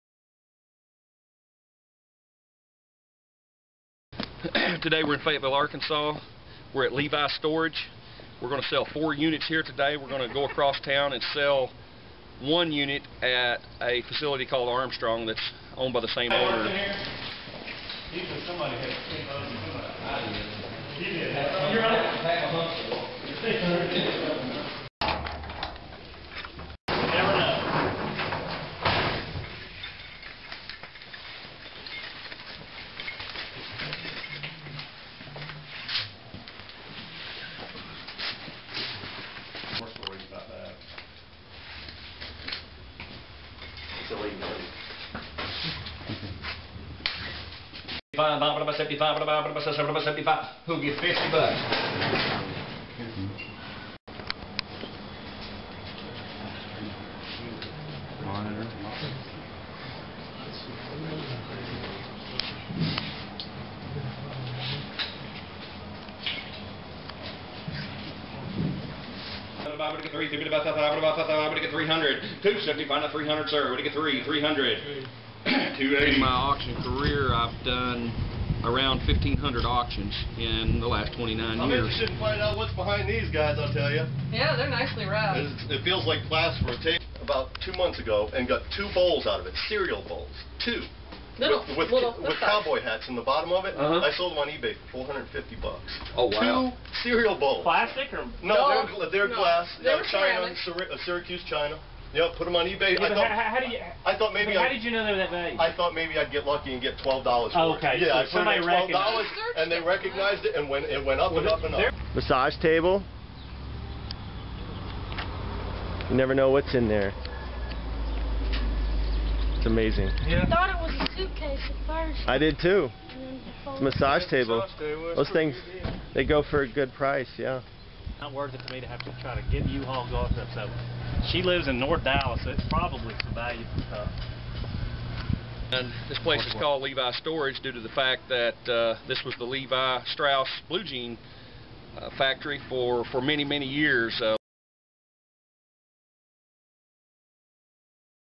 Arkansas Storage Auction
My opportunity to film an actual storage auction with an authentic Arkansas auctioneer!